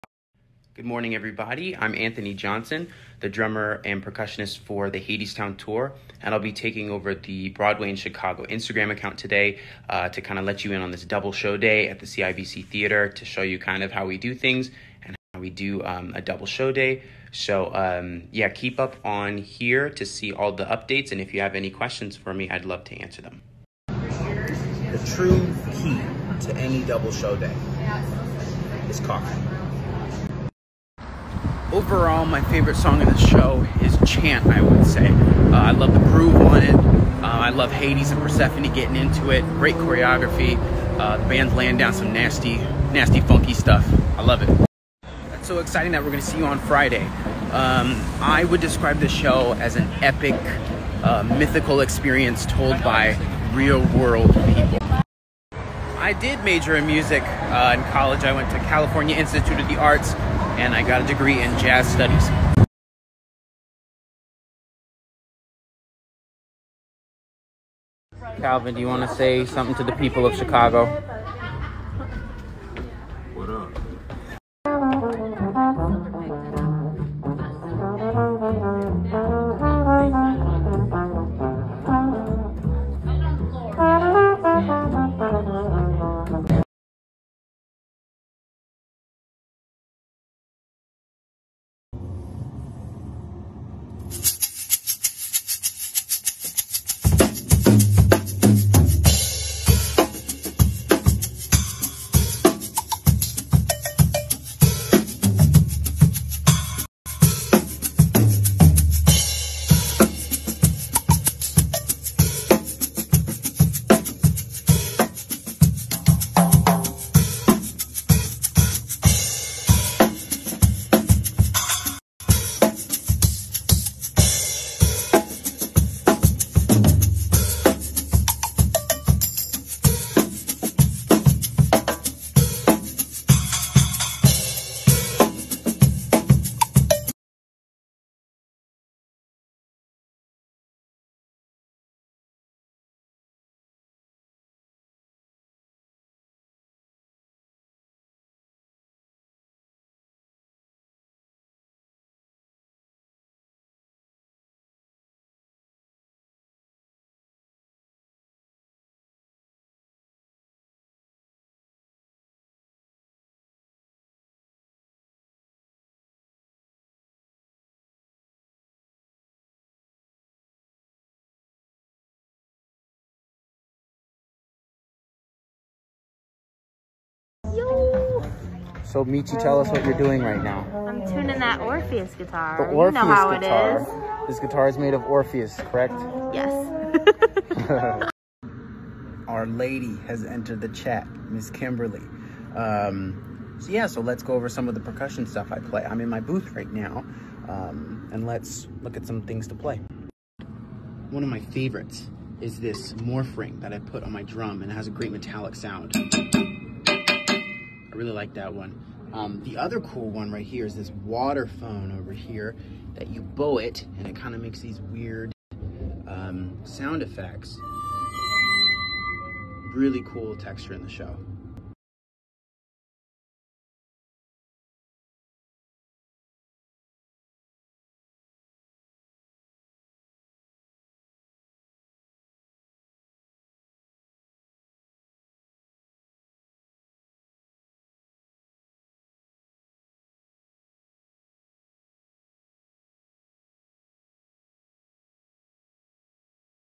Hadestown | Anthony Ty Johnson
Join percussionist from Hadestown, Anthony Ty Johnson, as he takes us through a 2 show day!